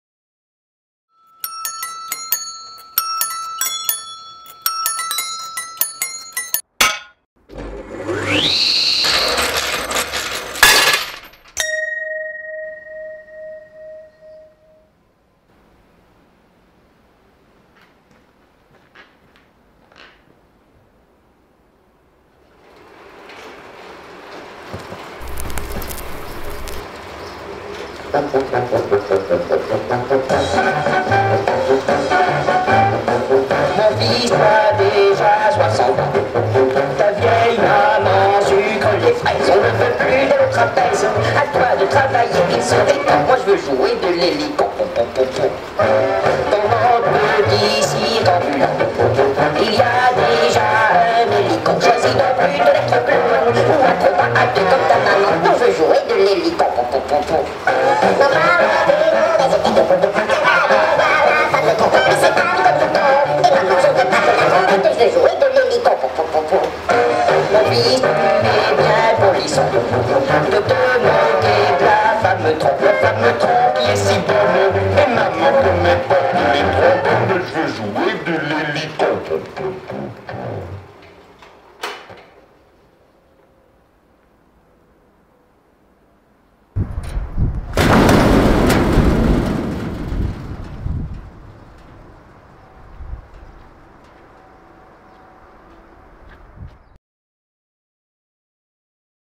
La chanson du vendredi #3
Du lundi au vendredi, tous les jours à 11h, entrez dans l'univers désespérément loufoque des artistes-ouvriers de la compagnie de théâtre de rue Dynamogène (Nîmes)

Avec Grégoire De Martino, Pepe Martinez et Pierre Pélissier + invités
Réalisé par Pepe Martinez
Dynamogène Productions